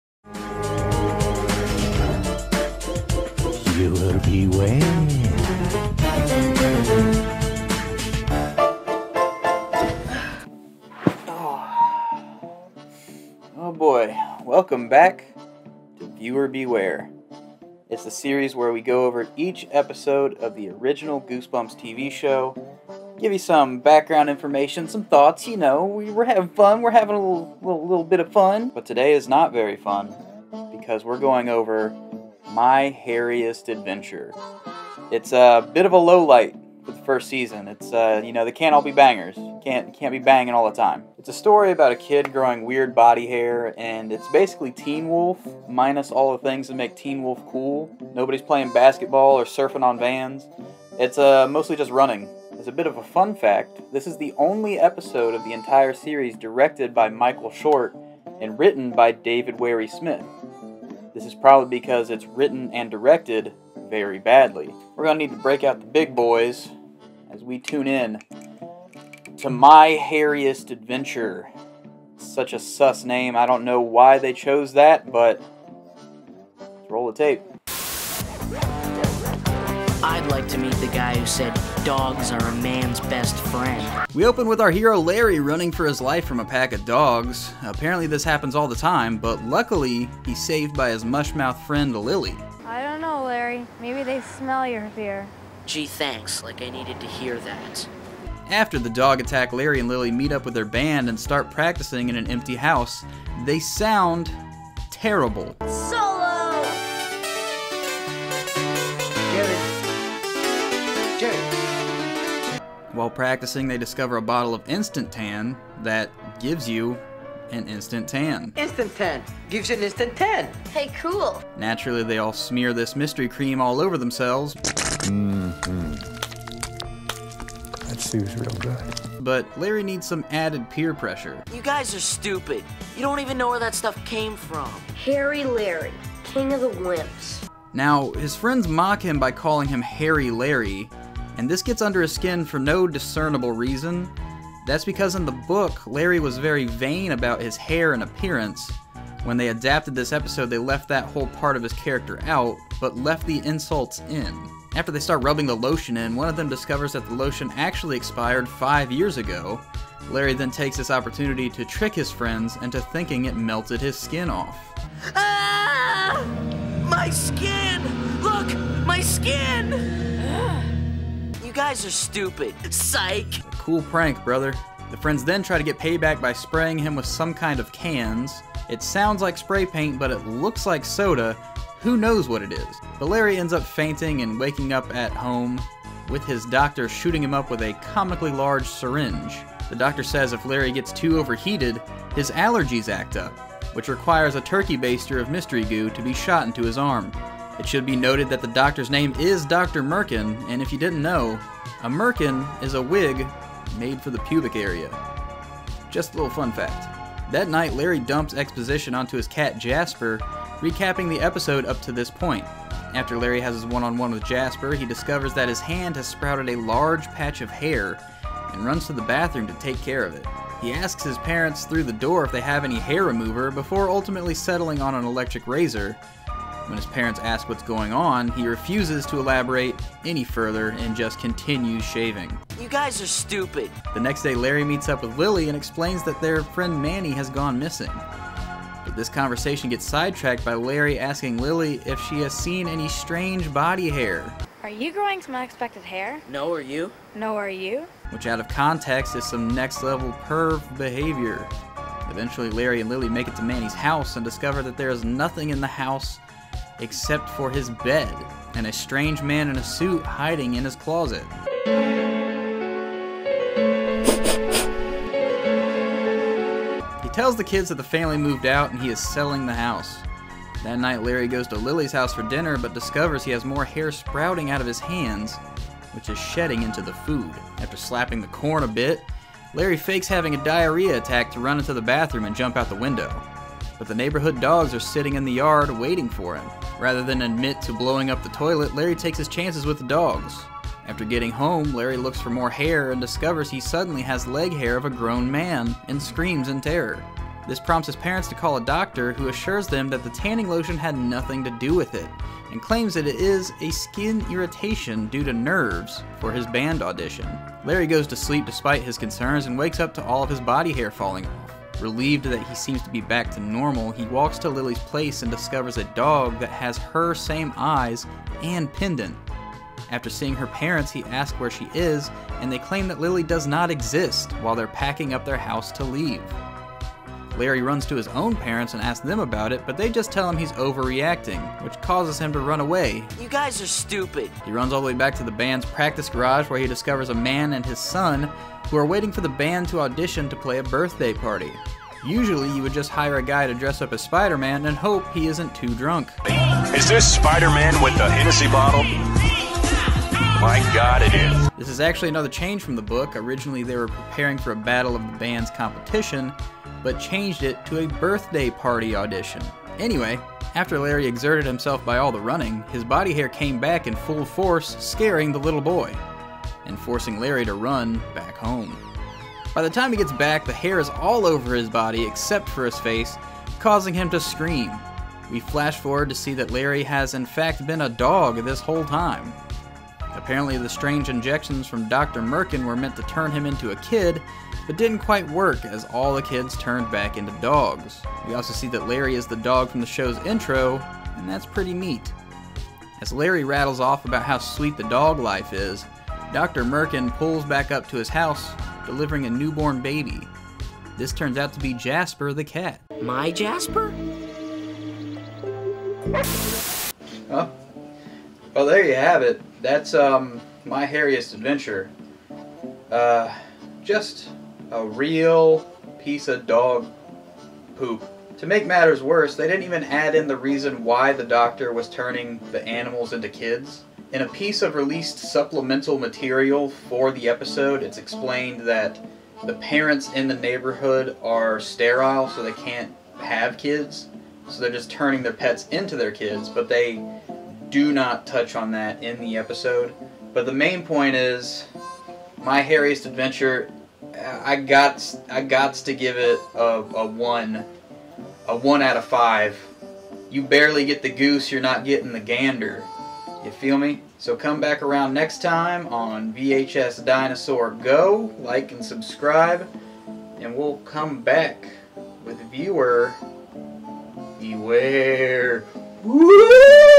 Viewer Beware - My Hairiest Adventure - Goosebumps Review
On tonight's episode of Viewer Beware, we are checking out My Hairiest Adventure, one of the worst episodes of Goosebumps. We get to see strange body hair, bad lip dubs, worse music, and some sweet sun tan lotion.